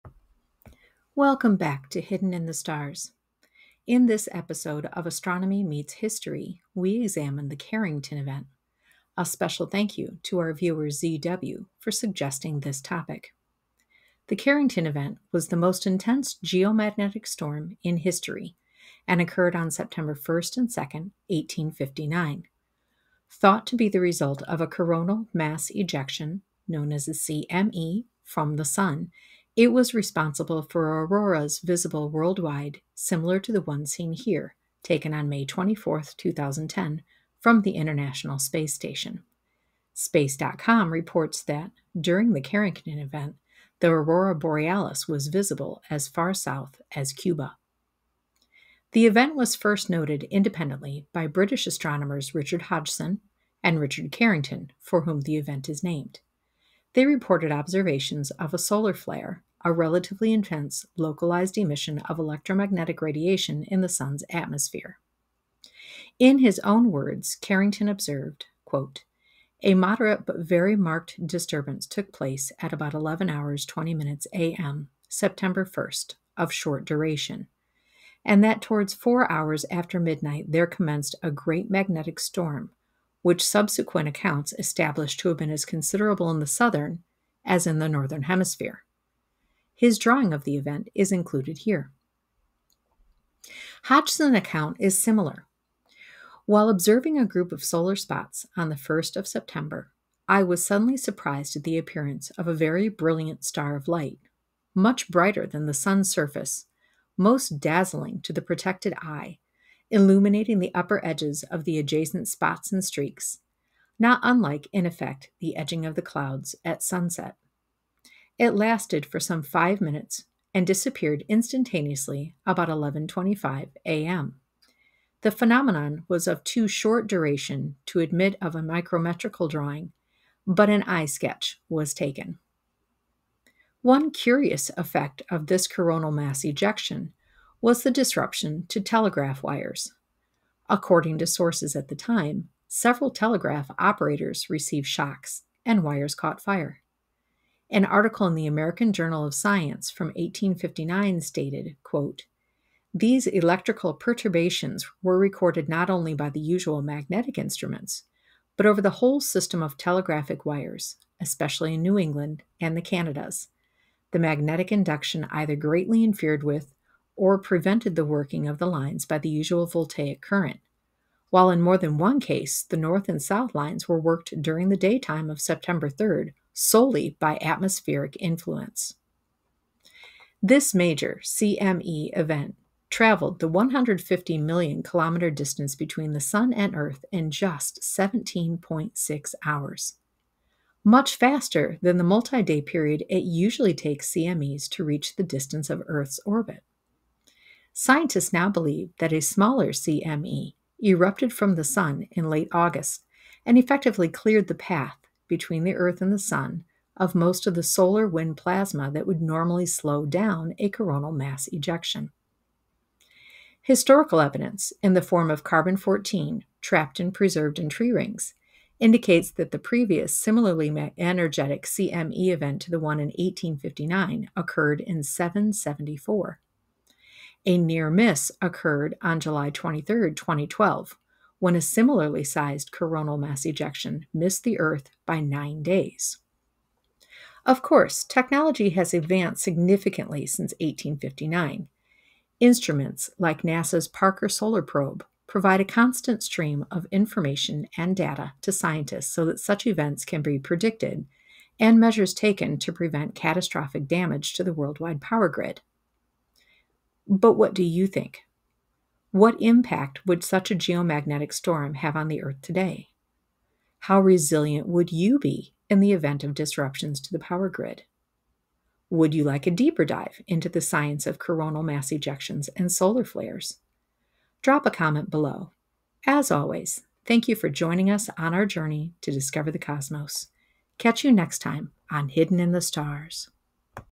The Carrington Event
It's hard to imagine a power strong enough to disrupt communications worldwide, but coronal mass ejections (CME) from the sun are capable of doing just such a thing. In this episode of Hidden in the Stars, we examine the Carrington Event, in which a CME interfered with telegraph lines. What effect would such an event have on our lives today? Watch to find out.

Carrington, R. C. (1859). "Description of a singular appearance seen in the Sun on September 1, 1859". Monthly Notices of the Royal Astronomical Society. 20: 13–15. Bibcode:1859MNRAS..20...13C. doi:10.1093/mnras/20.1.13.

Hodgson, R. (1859). "On a curious appearance seen in the Sun". Monthly Notices of the Royal Astronomical Society. 20: 15–16. Bibcode:1859MNRAS..20...15H. doi:10.1093/mnras/20.1.15

Loomis, Elias; Kingston, G. P.; Lyman, C. S.; Twining, Alexander C.; Kirkwood, Daniel; Cornette, A.; Poey, Andreas; Trask, John B.; et al. (1859). "The great Auroral Exhibition of August 28th to September 4th, 1859". American Journal of Science. 2nd series. 28 (84): 385.